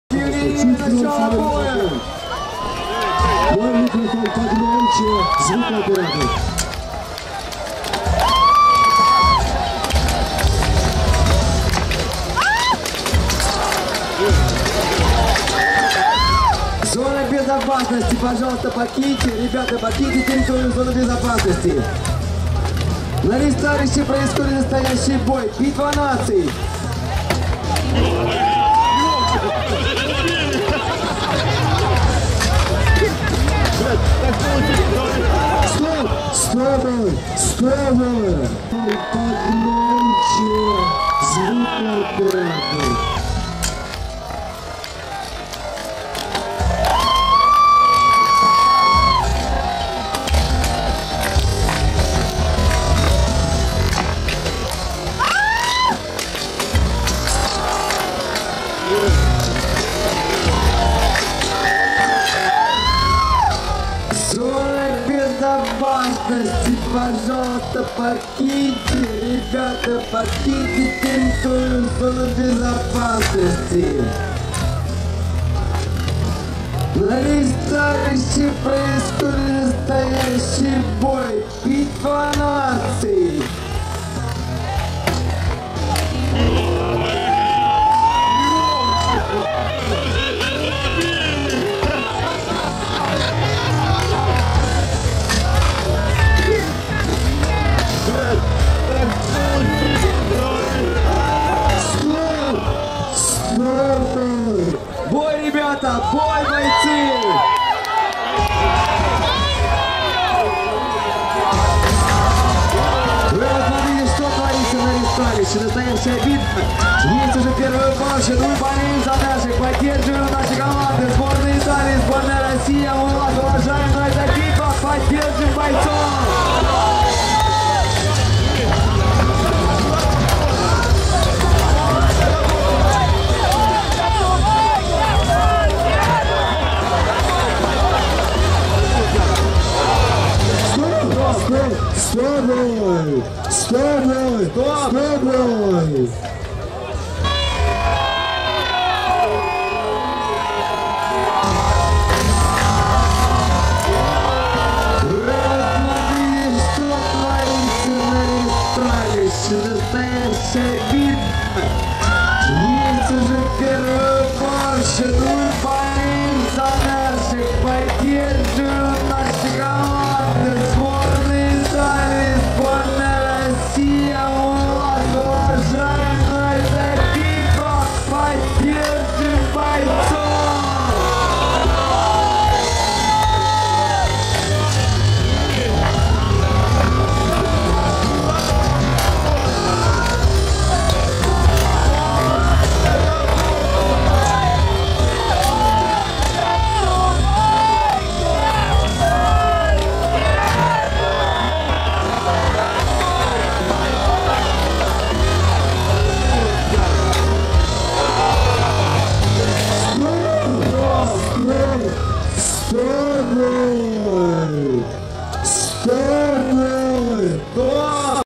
Battle of the Nations_21x21_Poland-Russia_4_бой_02.05.2011.mp4
Видеоматериал любезно предоставлен Юрием Валентиновичем Спиридоновым.

"Битва Наций" - 2011. г. Хотин, Украина.